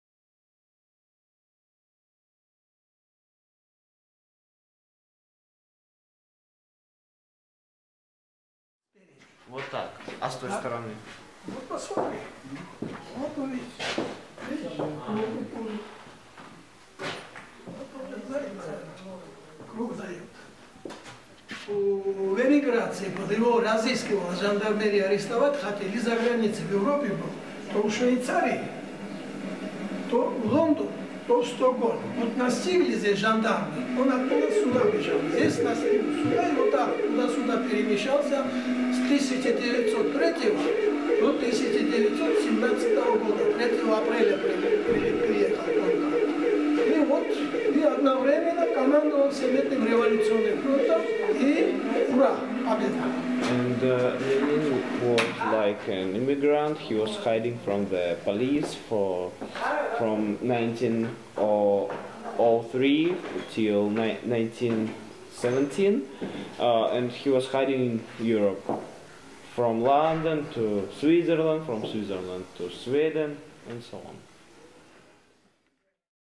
Joseph Stalin Underground Printing House Museum Tbilisi
These are a few images from inside the odd Joseph Stalin Underground Printing House Museum. It's like a real time warp into the Soviet past. And Zhuli the guide still believes. This video was made to accompany a more extensive essay which can be found at The Anadromous Life.